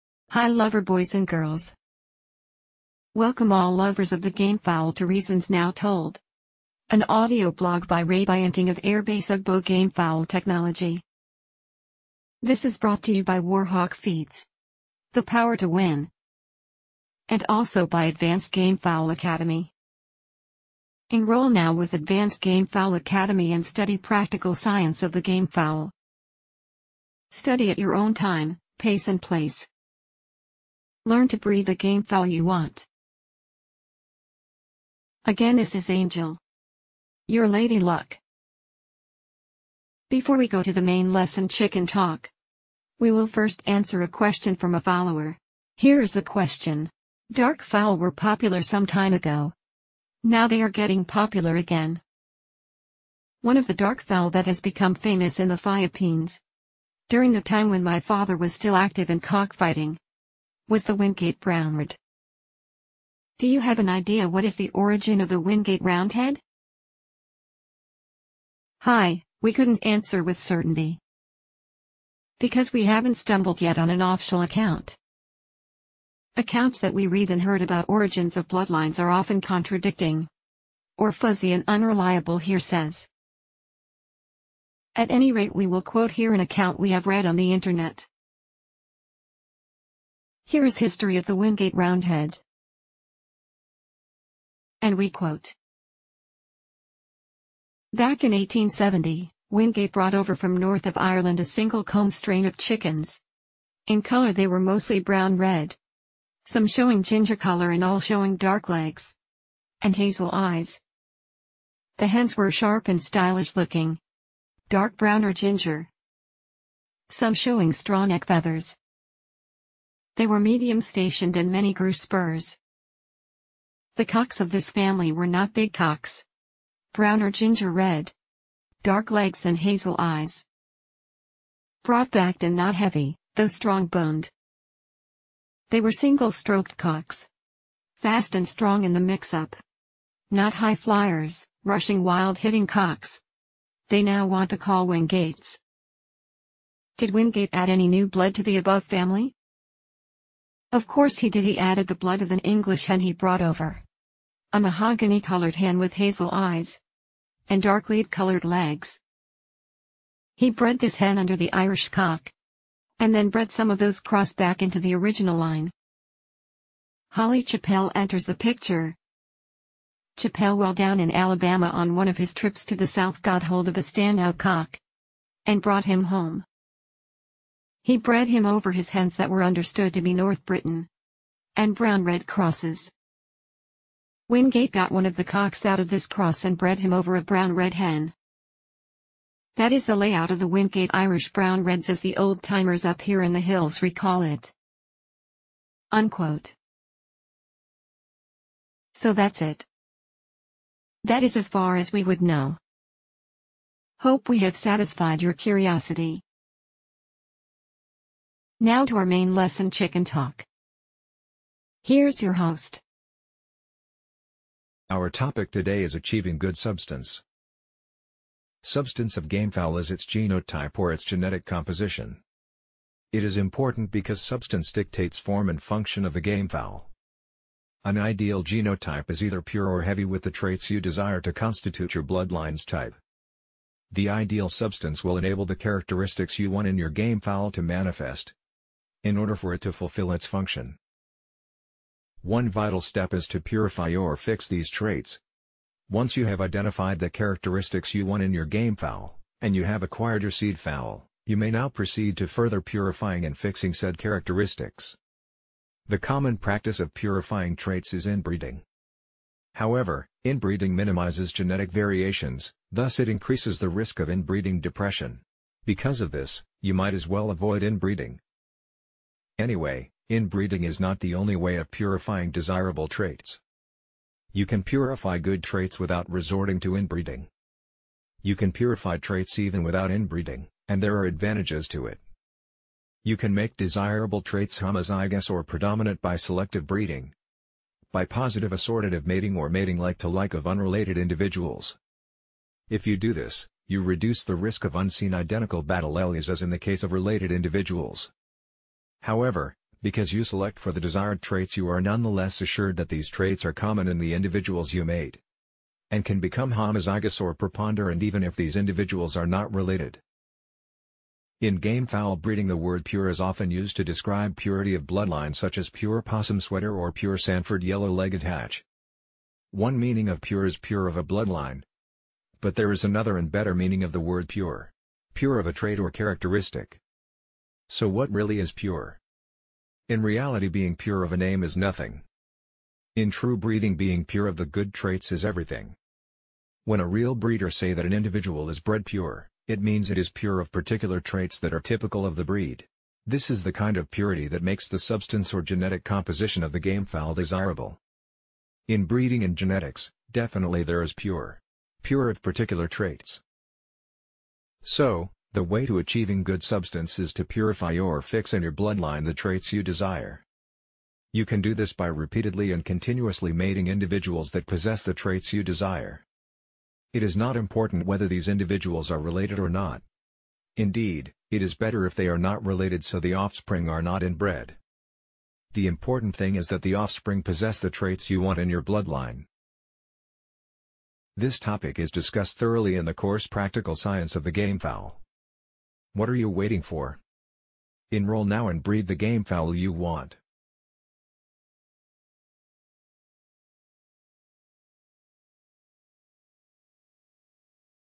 RNT 17 IDEAL SUBSTANCE
An ideal genotype is either pure or heavy with the traits you desire to constitute your bloodline’s type. The ideal substance is one that will enable the characteristics you want in your gamefowl to manifest in order for it to fulfill its function.

Therefore you aim for seed fowl for the traits you want. You want breeding materials with the characteristics you desire. We can’t make a seed fowl by breeding any pure of a bloodline to another pure of the same bloodline. Nor can we make a seed fowl by continuously inbreeding members of just any family. We can only make a seed fowl by continuously and repeatedly pairing individuals that possess the traits we desire, pure or not, inbred or not, related or not these individuals might be.

For example by breeding individuals with high flying ability we may end up with a bloodline with members possessing high flying abilities. Same with consistently breeding power to power or speed to speed. The end result may be individuals heavy with those traits.  Of course, when it comes to fighting ability, this principle is easier said than done. Fighting ability is polygenic, dictated by so many different genes.  For simple traits, we can aspire for pure. For traits that dictate fighting ability we cannot. An individual heavily packed with genes of the desired characteristics would suffice as a seed fowl for these characteristics. In polygenic and quantitative traits we aim for preponderance of positive traits. Therefore, it is not a plausible goal to have a bloodline heavy with all the traits that make a good fighting rooster.

In short you do not aspire for a seed fowl for all the traits. You create seed fowl for some particular traits. Then, breed these seed fowl with one another and hope that their respective traits will converge in your battle fowl.